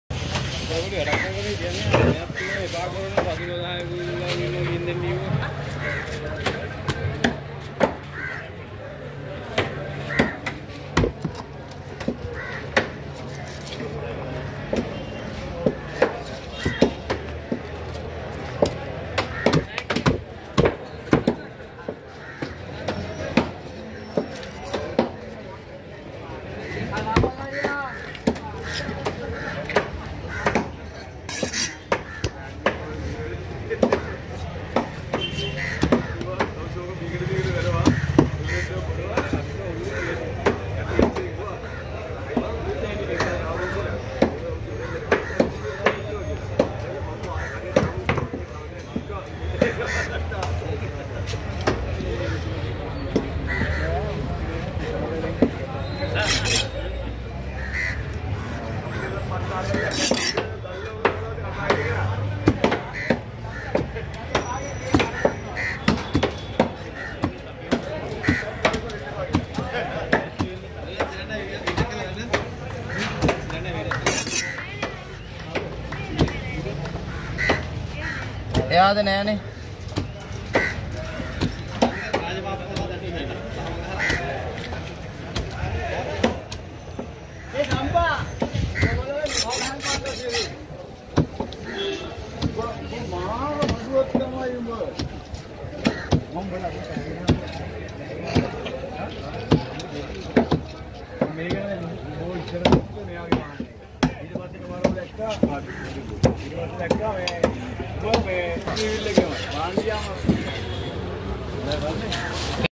Amazing Seer Fish Cutting Skills Expert Cutting Seer Fish In Sri Lanka Fish Market
King Seer Fish (Vangaram)fish cutting and chopping skills by an expert fish cutter at Lellama fish market in Sri lanka. Enjoy professional fish cutting techniques by the fish cutting expert. I hope, you will enjoy this fish cutting video.